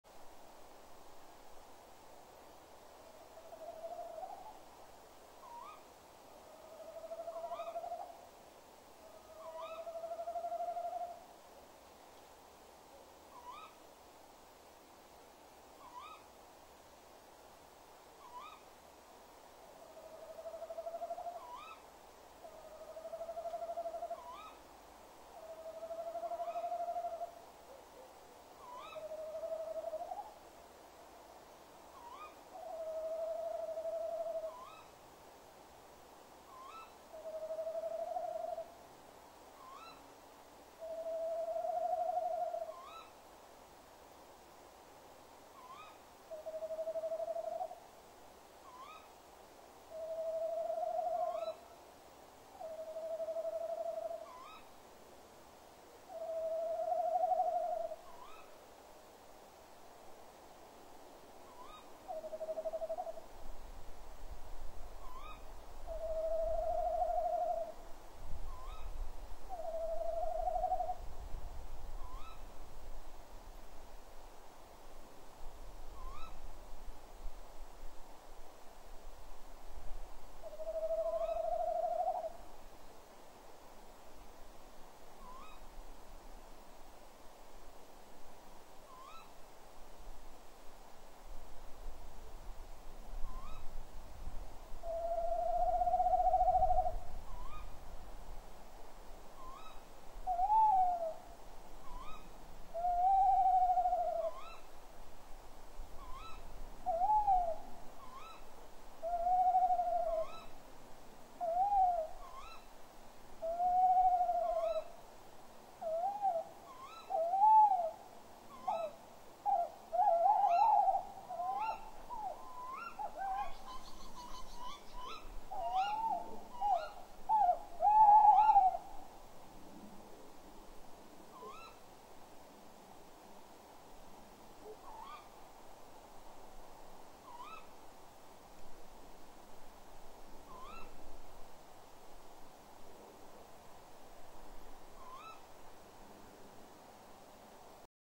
BosCam1 2016 01 20 195620 Bosuil M bij de kast V in de buurt goed gesprek
19.56 uur. De uilen roepen en M komt bij de kast. Hij rolt zeer overtuigend en gaat dan over op heel enthousiast roepen, terwijl V antwoord geeft.. Hij vliegt weg en V blijft het langste roepen.